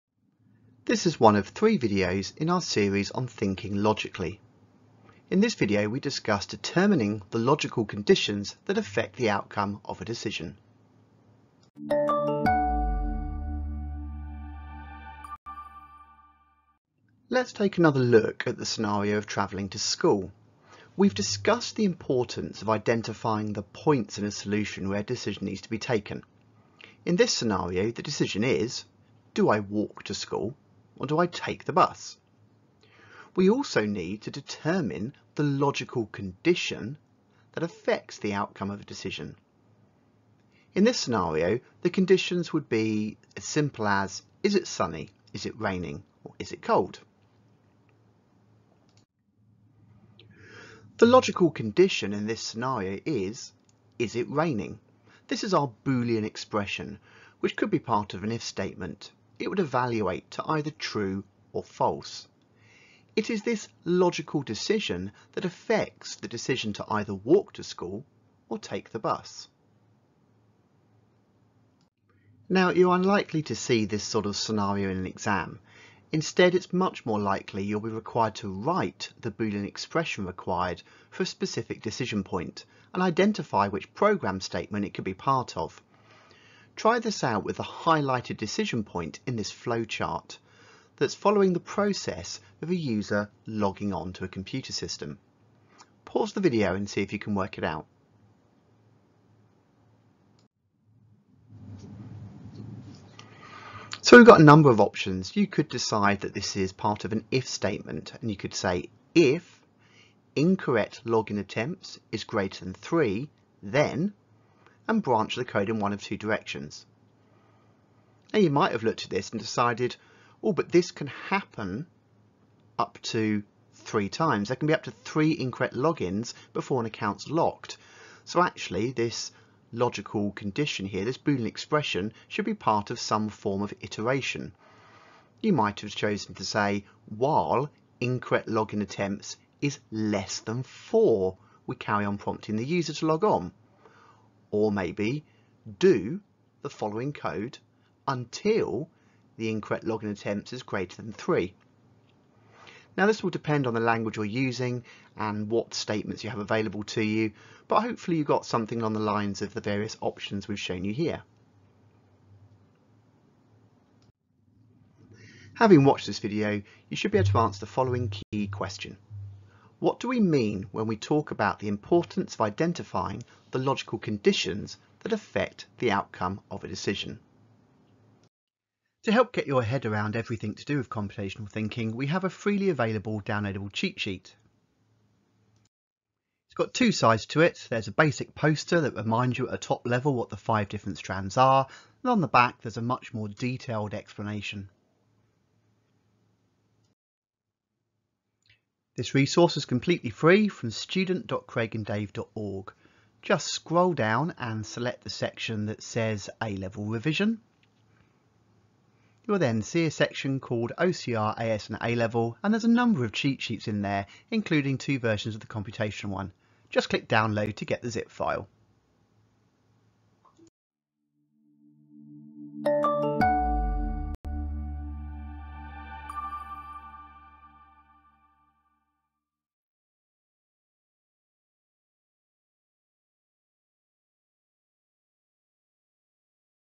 129. OCR A Level (H046-H446) SLR21 - 2.1 Determine logical conditions
AS Level 2.1.4b
A Level 2.1.4b

00:00 Intro
00:17 Determining Logical Conditions: A Scenario
01:12 Boolean Expression Example
02:44 Key Question
02:58 Computational Thinking Cheat Sheet
03:41 Outro